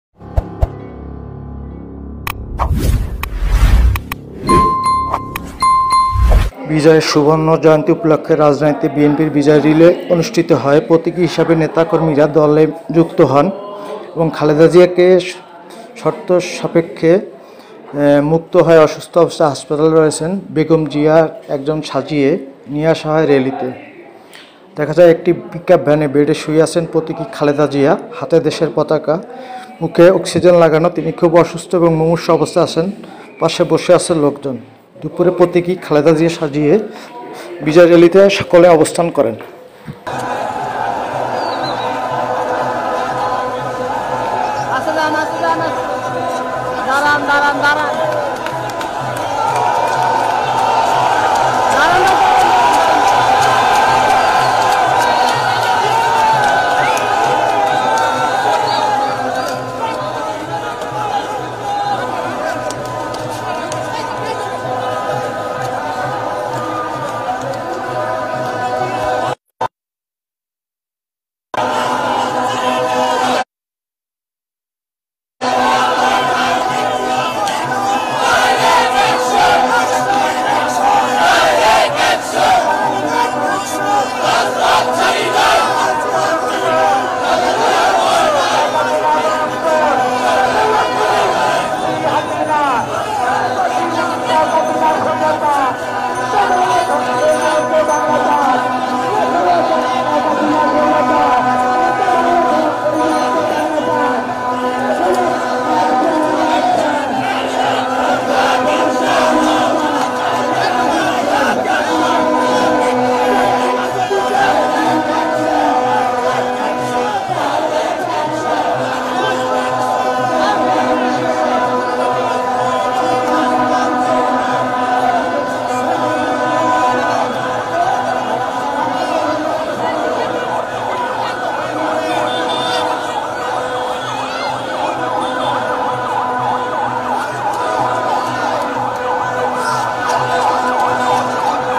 বিজয় দিবসের মিছিলে রাজপথে প্রতীকী খালেদা জিয়া
Video Copyright: Bangladesh National Party BNP Facebook Page.

Thanks